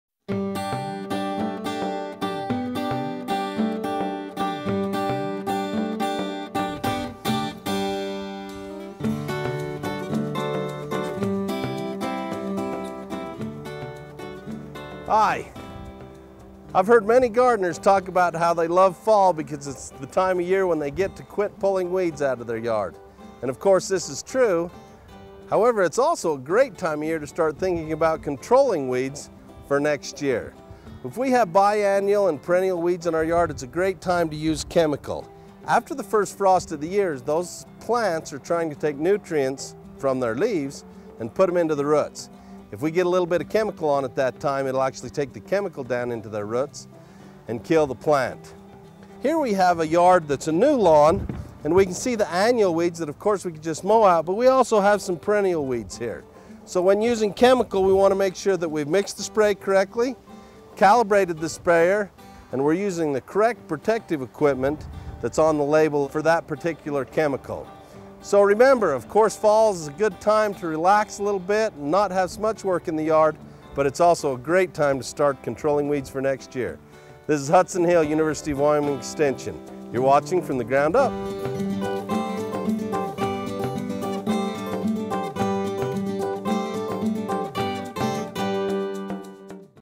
Fall Weed Control | From the Ground Up
Even though gardening and lawn care are winding down for the year, effective fall weed control can help you get a jump start on next year's weeds.

Horticulture